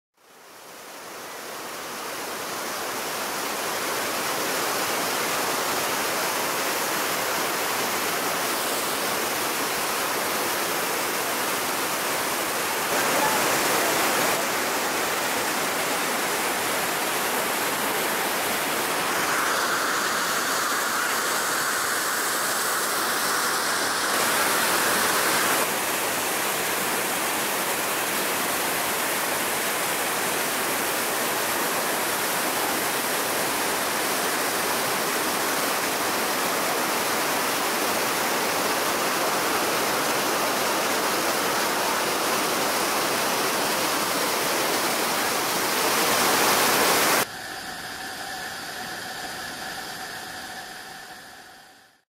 Tout près de la grande chute d'Iguaçu côté brésilien - Iguazu Falls, Brazil side
Tout près du bord de la chute, le bruit est infernal ...